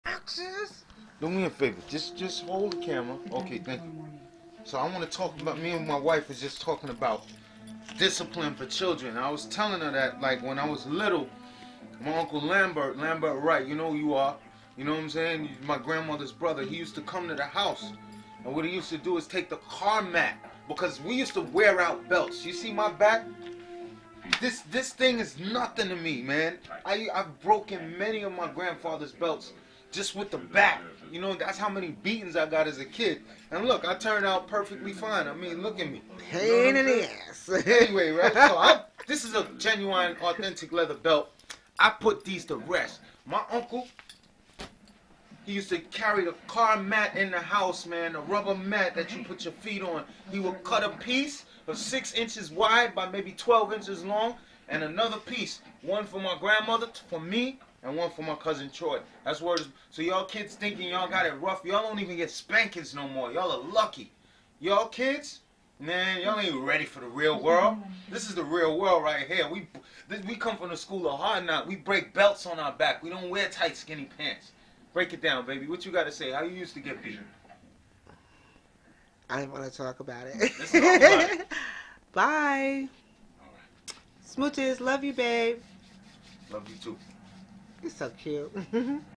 belt beating
is rubber more durable than leather?